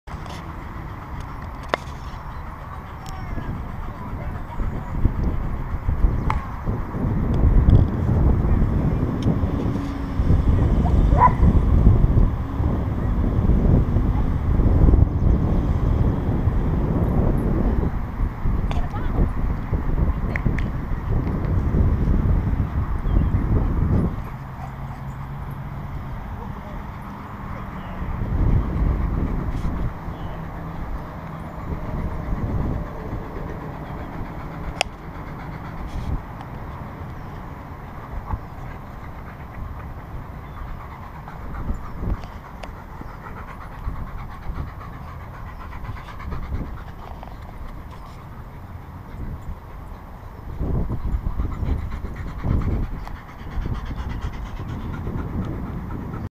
63KG英國獒犬+45KG荷蘭狼犬
1 year old English Mastiff and 5 year old Dutch Shepherd.